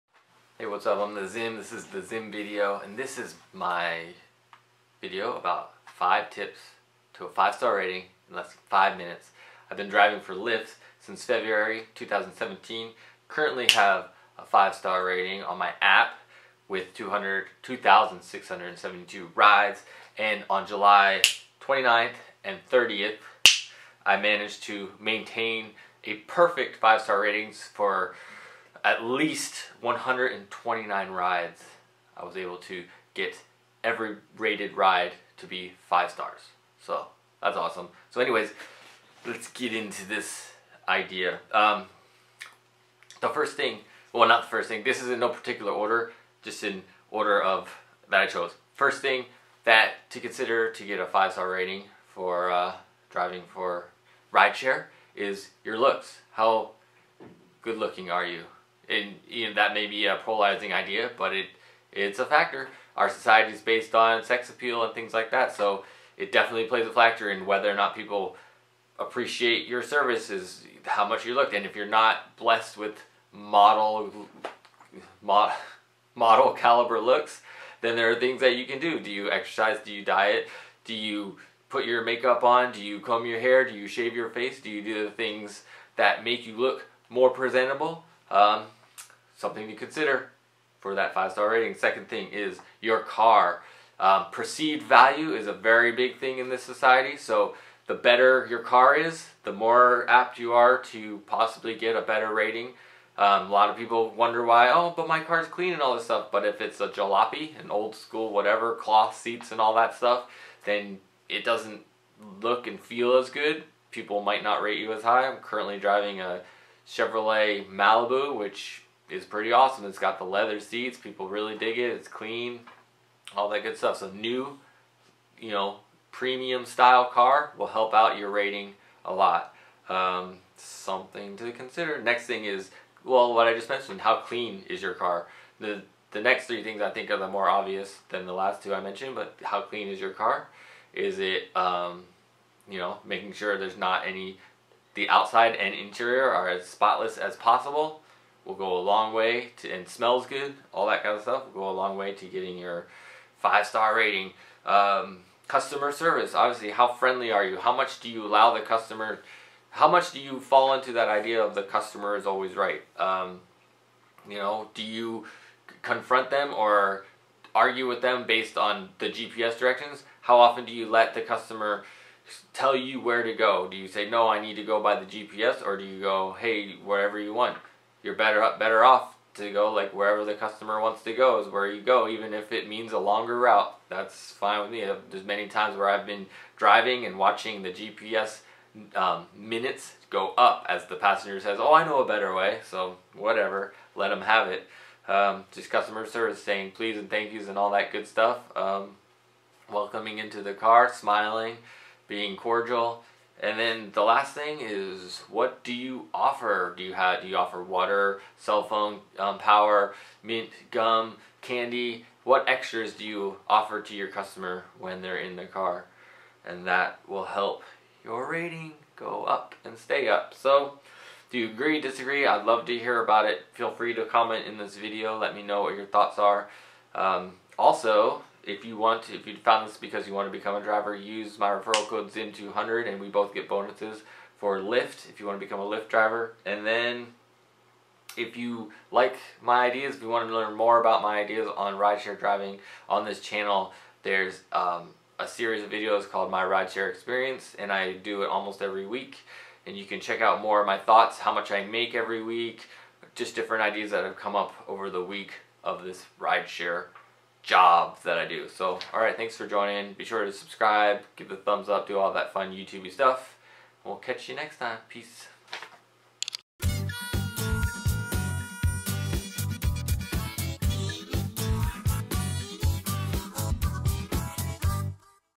5 Tips For A 5 Star Rating In 5 Minutes Driving For Lyft And Uber
This is a video about how to get and keep a 5 star ratting while driving for Lyft or Uber.

• Your Looks
• Your Ride
• Cleanliness
• Customer Service
• Extras

IG account about my rideshare exp @ridesharezim

Alexander Thomas